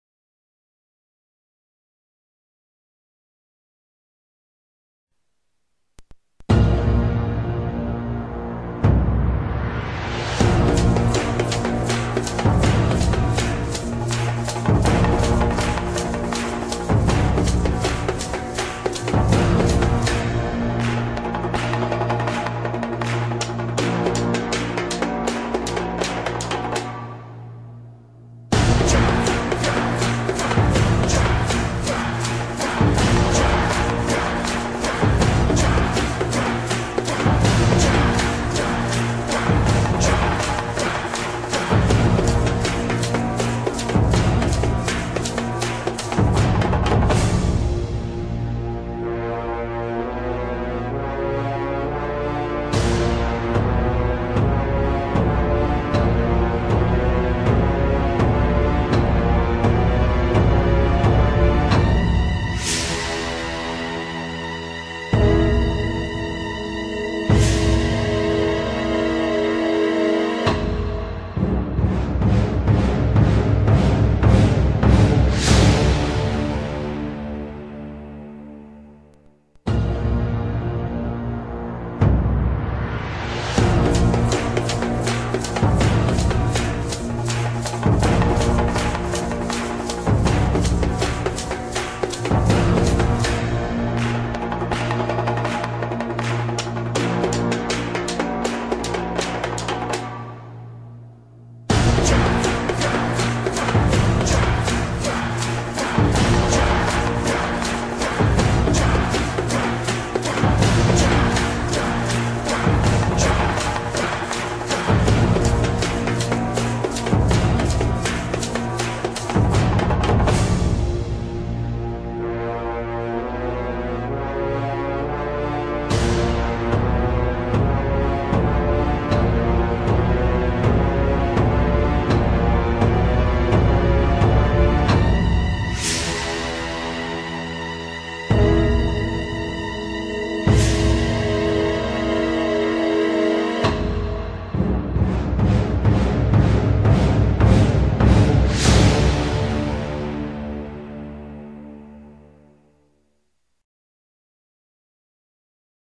shodans gradings.wmv
Four of our students and friends went for their shodan ,with great effort and dedication they passed the exam.Our 2nd woman Liz zammit ,Daniel camilleri ,the youngest Glenn Abela 16 yrs and the oldest my father Tony Camilleri at the age of 60yrs. this is a small gift for your work enjoy as I did. for further info mail at or visit our